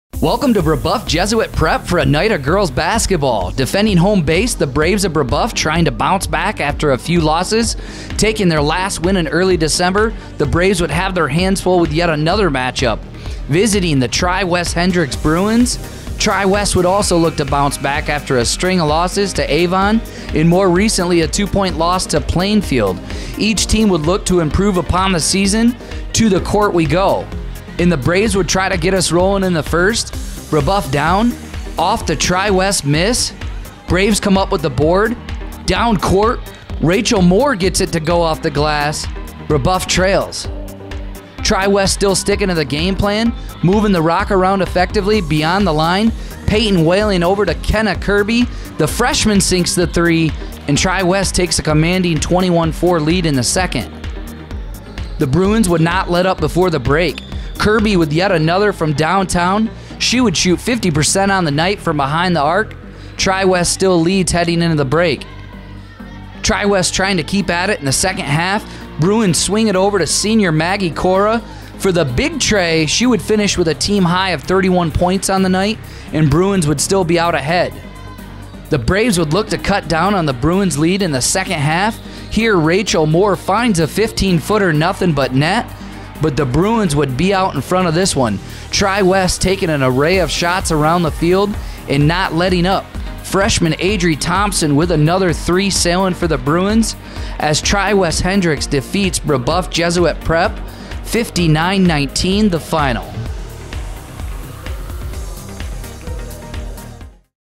Tri-West Hendricks vs Brebeuf Jesuit Prep | Girls Basketball | STATE CHAMPS! Indiana
Catch all the highlights of Tri-West Hendricks vs Brebeuf Jesuit Prep in Girls Basketball.